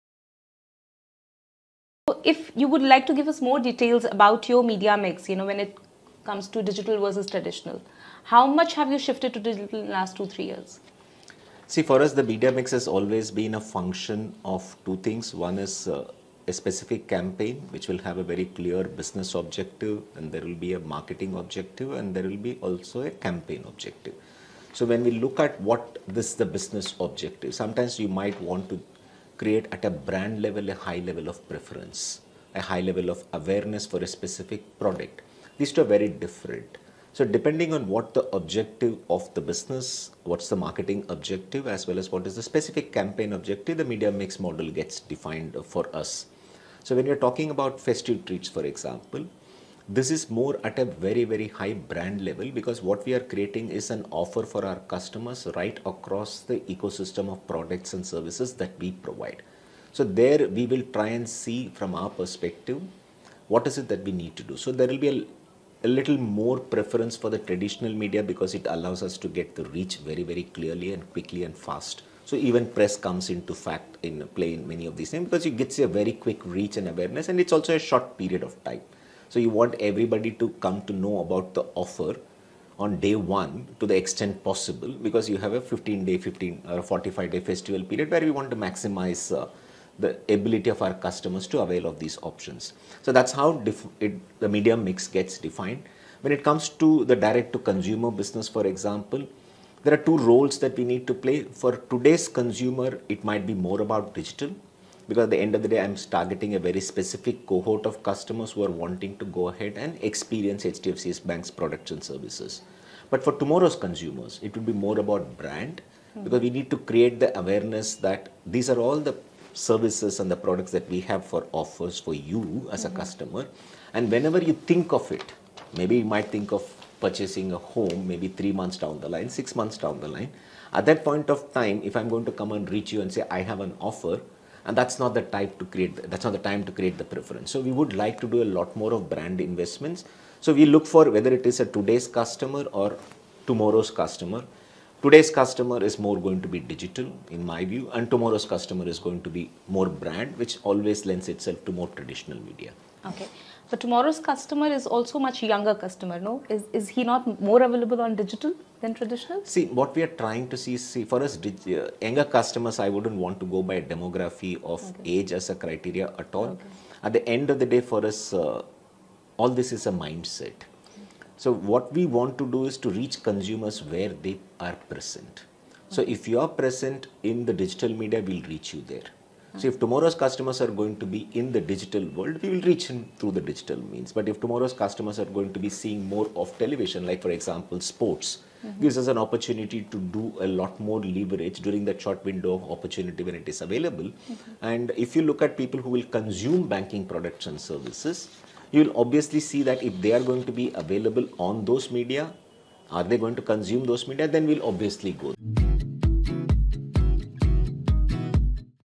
Ravi Santhanam, HDFC Bank talks about the Media Mix in terms of digital vs traditional.
"Today's customer is going to be digital in my view and tomorrow's will be more brand, which always lends itself to more traditional media." : Ravi Santhanam, Group Head, CMO, @hdfcbank talks about the Media Mix in terms of digital vs traditional.